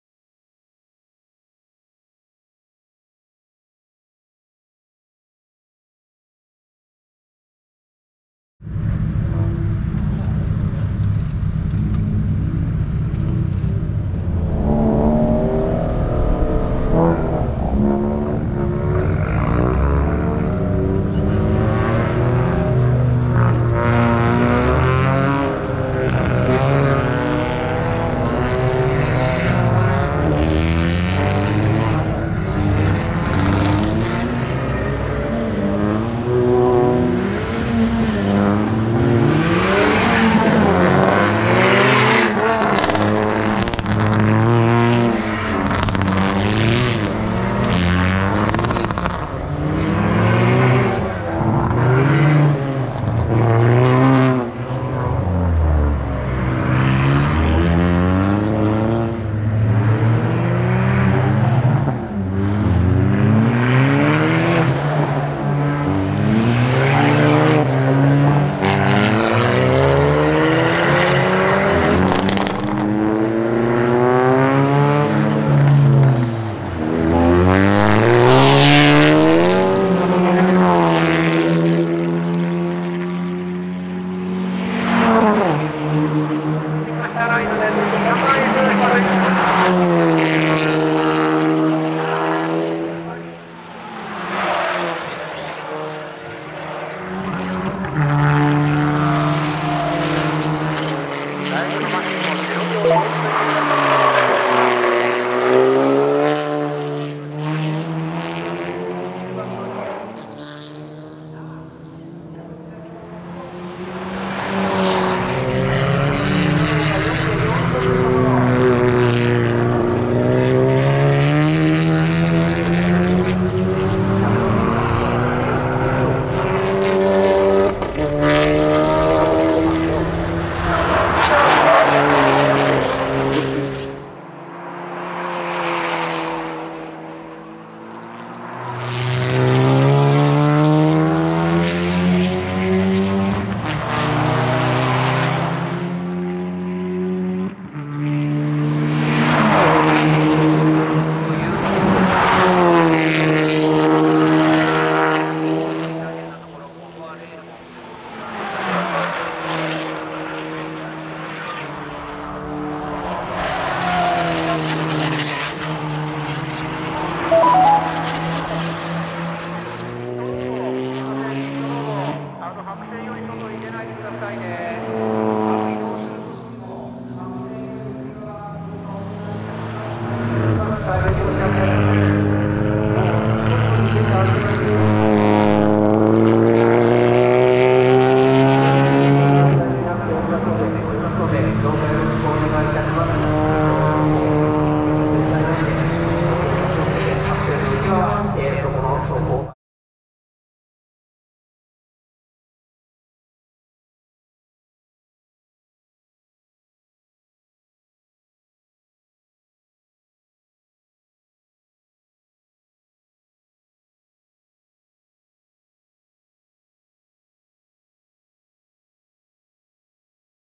Shibatire presents Attack Tsukuba 2024 ：RainbowAuto＠メタルZC33S/メタロー[Suzuki ZC33S]
シバタイヤ presents Attack Tsukuba 2024
-Shibatire presents Attack Tsukuba 2024
2024年02月17日（土）

スイフト SuperLap♪
No.170
RainbowAuto＠メタルZC33S
メタロー
Suzuki ZC33S

“★“印はベストラップ時ではない映像です。
The images marked with “★” are not the best lap times.